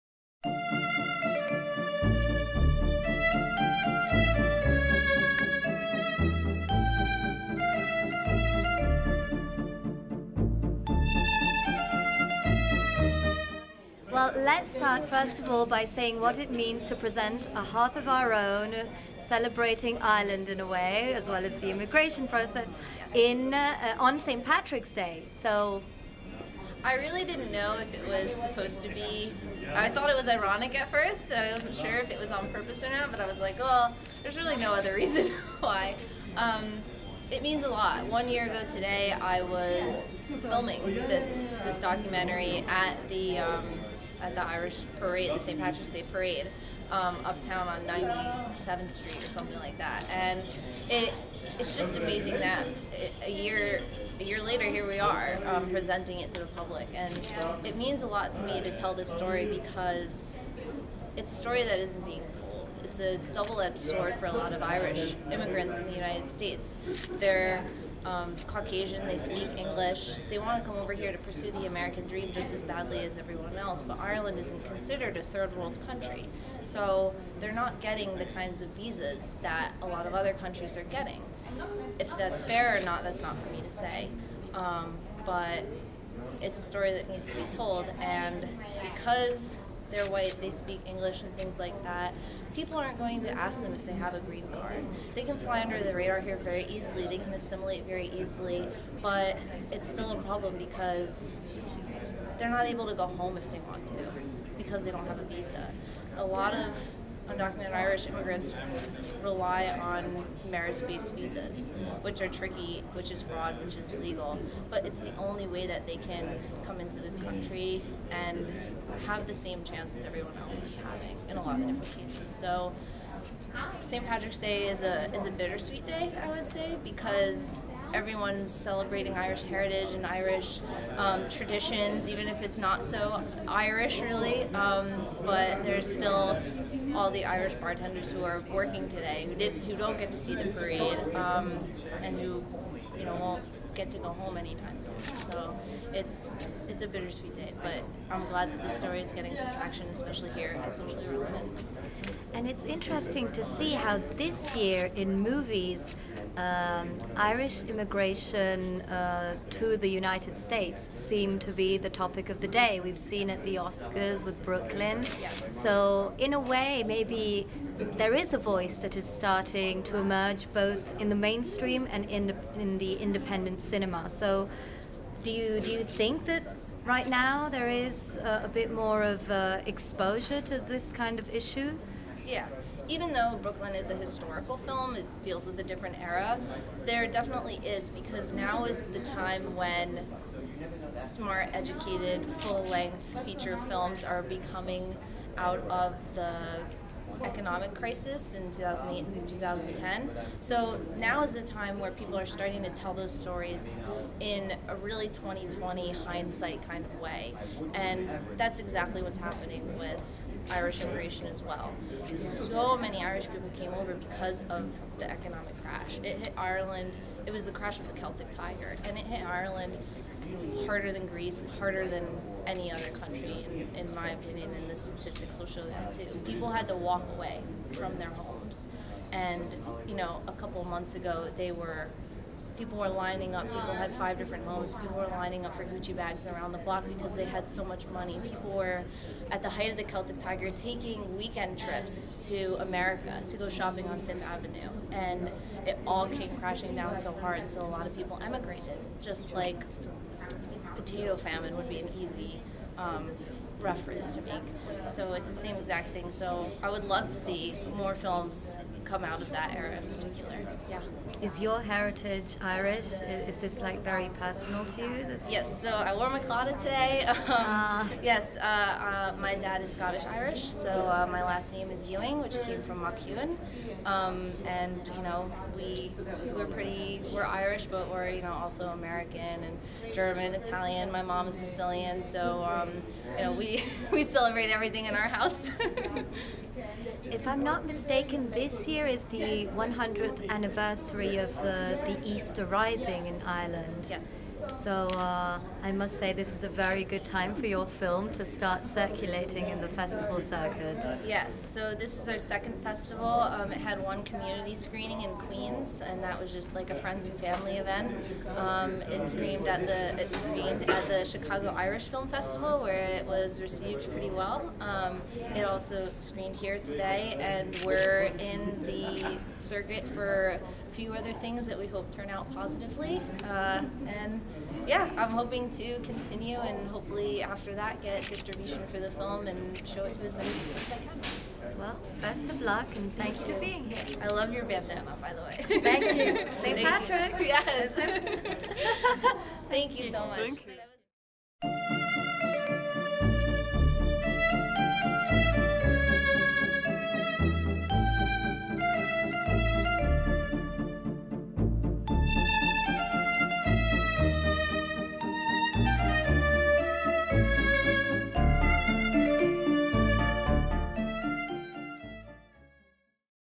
Interview A Hearth of our own
SRFF3, Interview with Gabrielle Ewing, Director A Hearth of our Own (USA/Ireland)

Forced out of Ireland after the economic collapse of 2008, Jacob is now one of the 50,000 undocumented Irish immigrants living in the US. He faces the threat of deportation unless he can marry an American girl - for paper, instead of love.